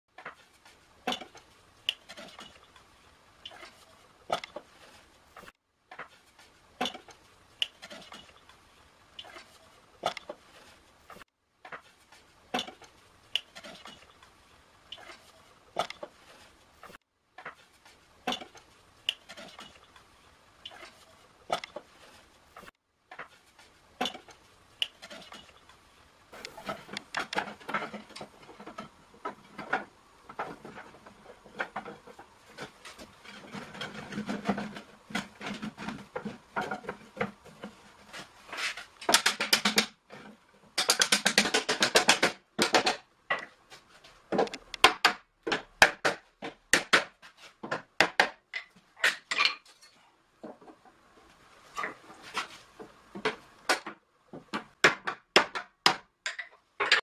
A whiskey barrel DIY. Assembling a wooden barrel...
How to make a whiskey barrel with your own hands. We make an oak barrel at home. Installing the bottom in an oak barrel.